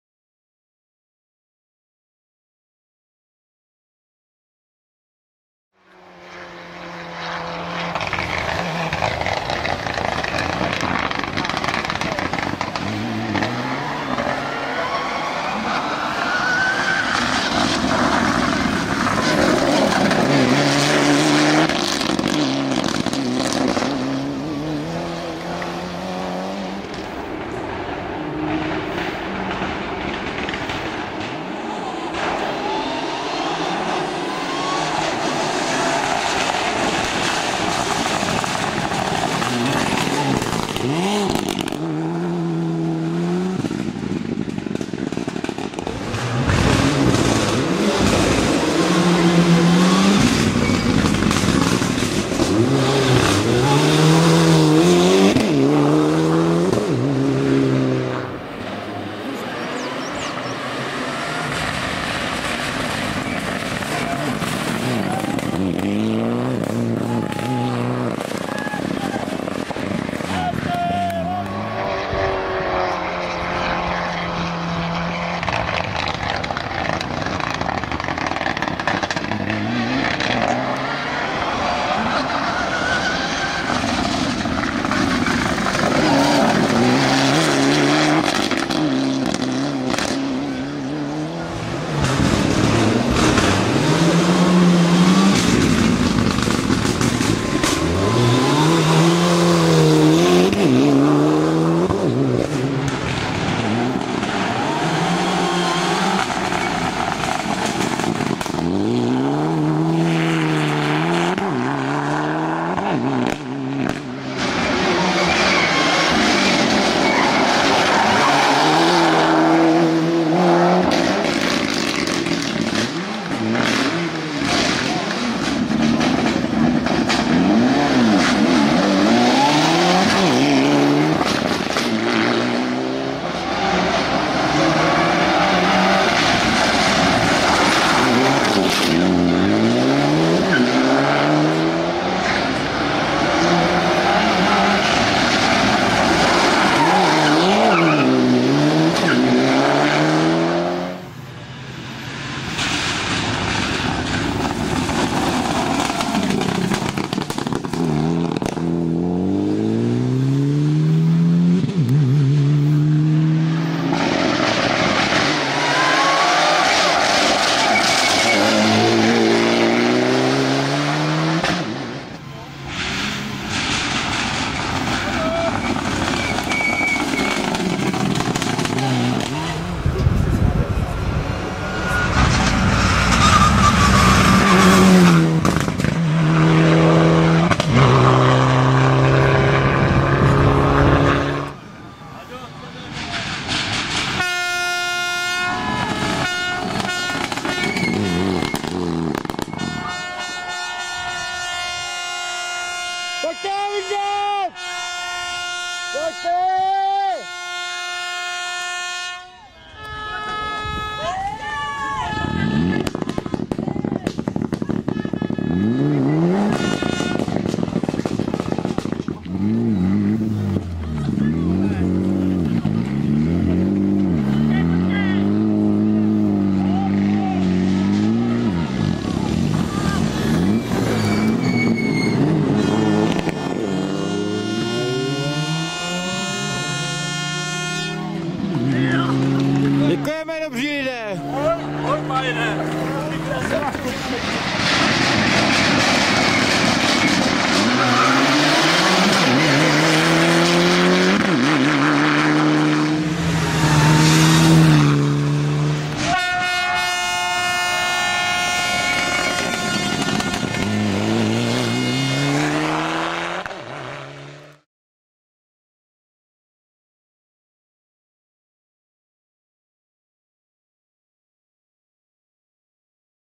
M. Lošťák - V. Kalný - Rally Pačejov 2024
Martin Lošťák a Václav Kalný s Mitsubishi Lancer EVO IX